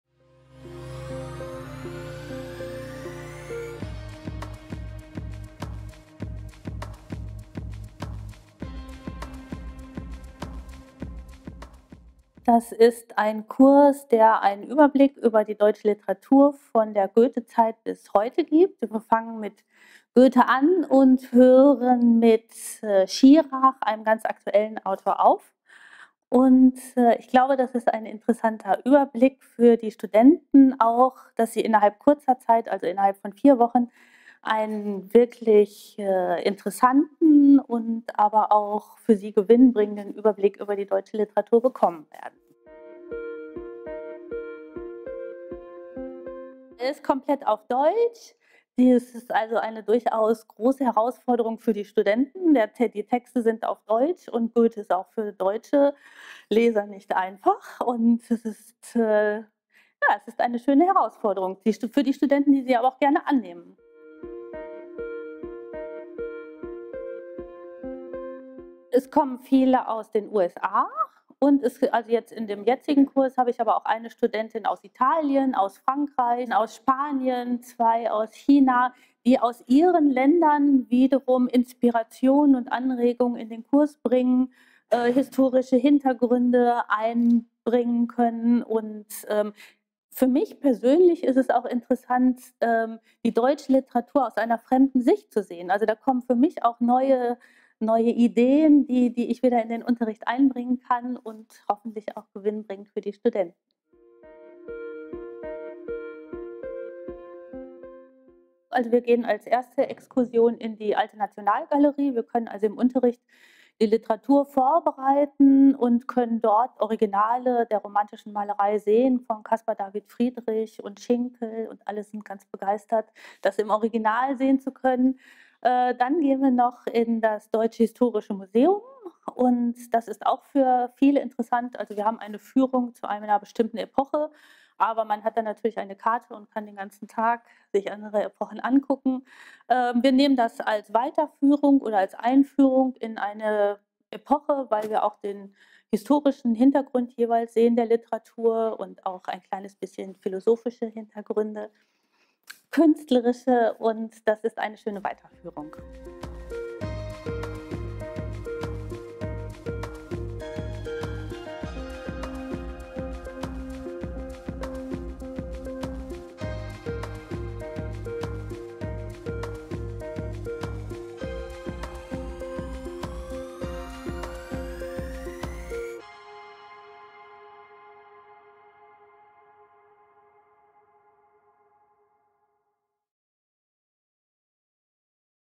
Key Works of German Literature with Dr. Anja Richter - FUBiS Talks
Get to know some of the instructors at Freie Universität Berlin International Summer and Winter University in the FUBiS Talks videos!

Dr. Anja Richter talks about her course "Schlüsselwerke der deutschen Literatur." The course is generally available online and on-site in Berlin - please check the FUBiS website for current offerings.

There, you can also register for FUBiS Term III (July 17 to August 14, 2021) until June 19.

Hope to see you at FUBiS soon - virtually or on-site in Berlin!